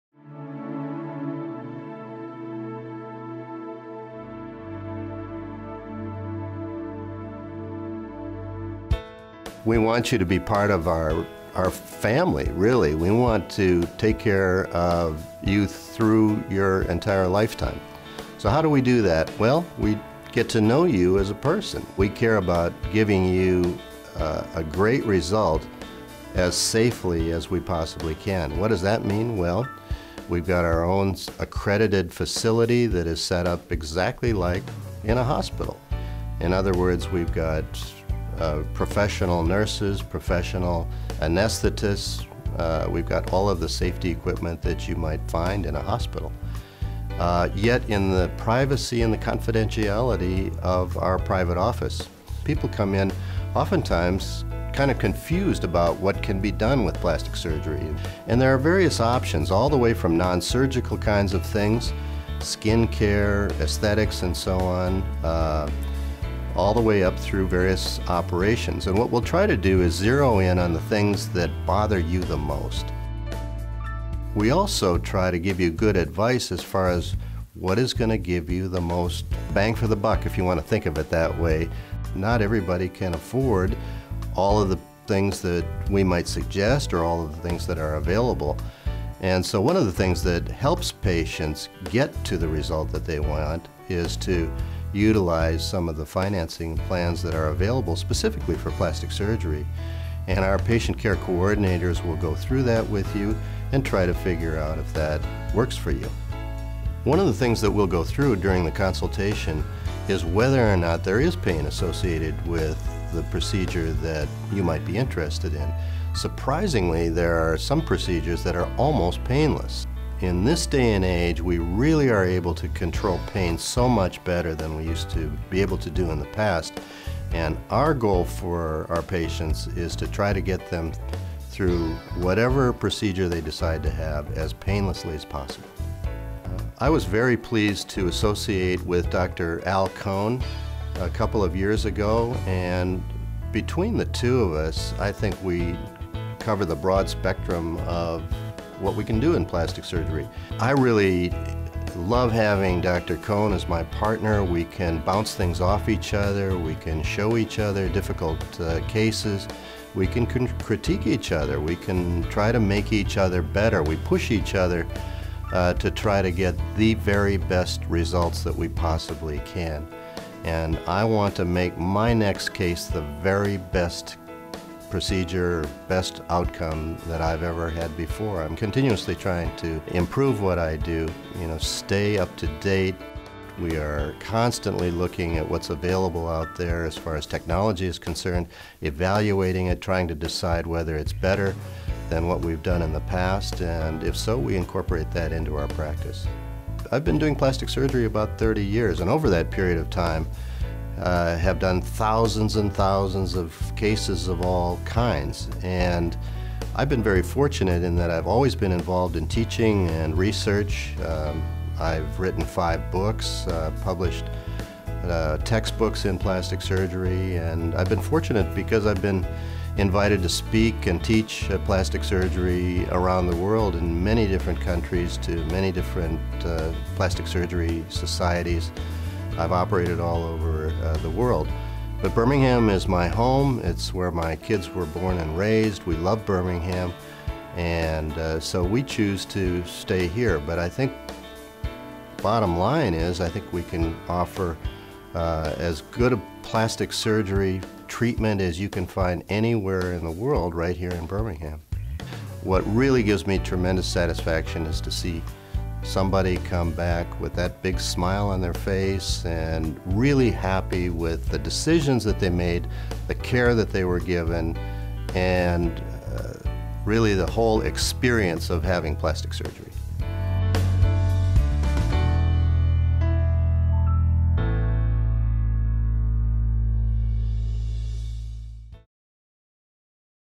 DR GROTTING FINAL
Dr. James C. Grotting addresses the philosophy of his practice and areas of special expertise.